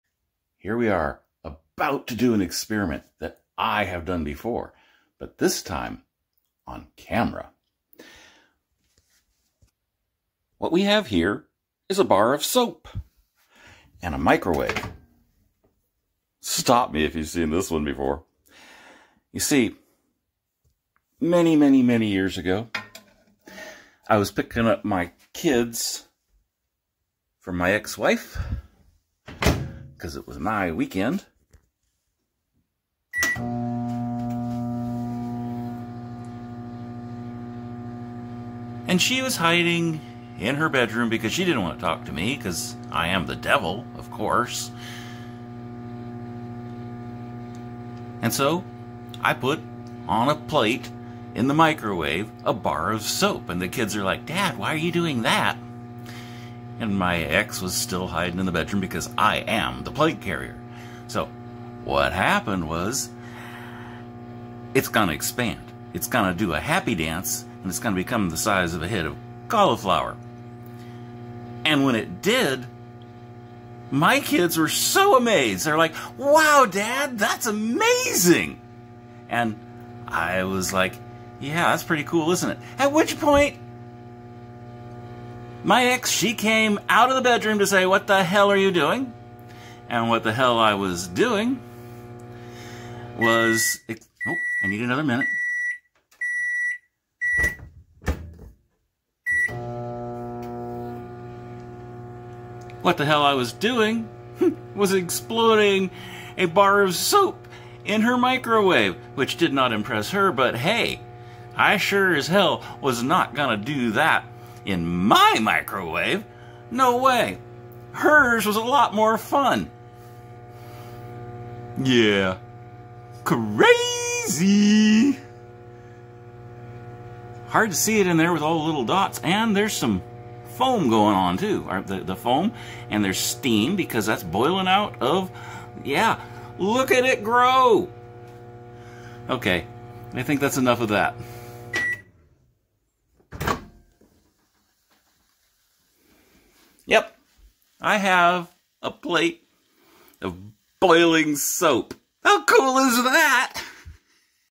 Microwaving Soap !
Classic Experiment!

Ivory hand soap
a minute and a half...

do it for a young audience, and you'll be cool for an hour!

I'm a Silverback nerd living among rednecks with mirrors on my roof and critters in my yard. And science on my counter! (It's Alive!)